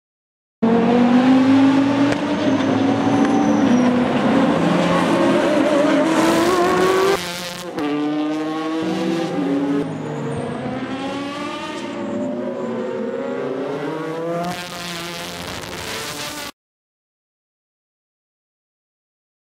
スーパーGT 2012 GT300 R'Qs Vemac 350R 公式テスト 岡山国際サーキット 2012.3.18
2012年3月17･18日に岡山国際サーキットで行われたスーパーGT公式テストの様子です。GT300クラスに参戦しているR'Qs Vemac 350Rのみの走行映像をまとめてみました。あまり撮影できていなかったので微妙な映像ばかりですが。。音割れも酷いのでボリュームを下げ気味で編集してます。
2012年3月18日撮影